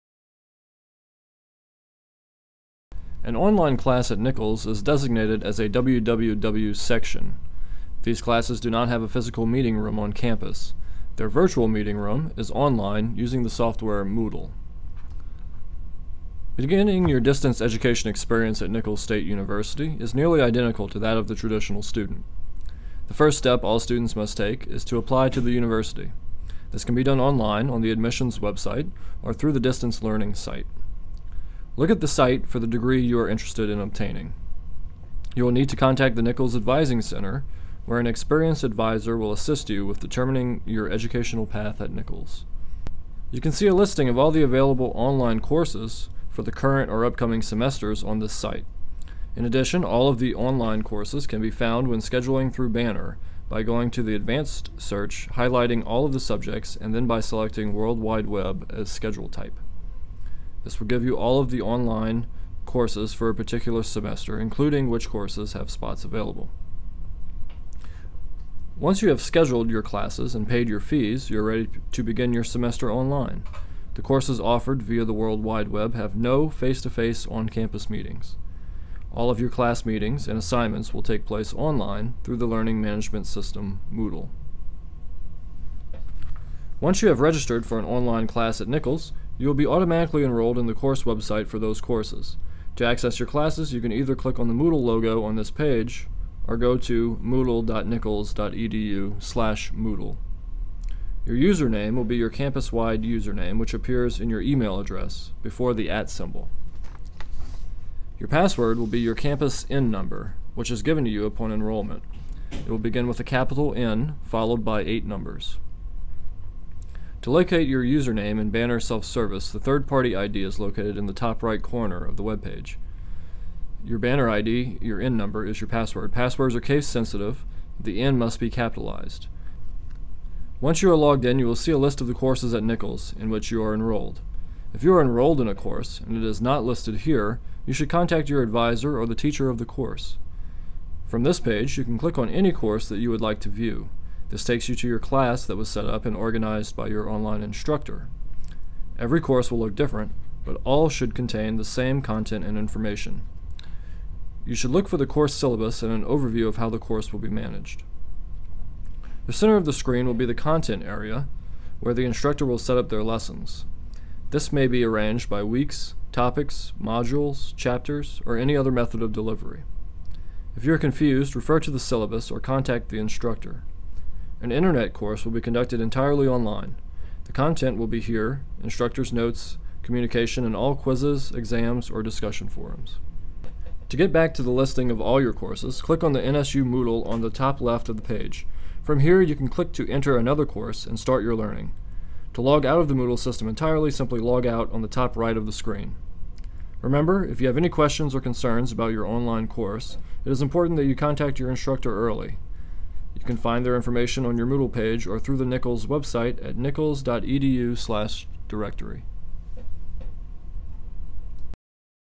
How to use Moodle
A quick and easy tutorial on how to use Moodle at Nicholls State University. Created by the Office of Distance Education.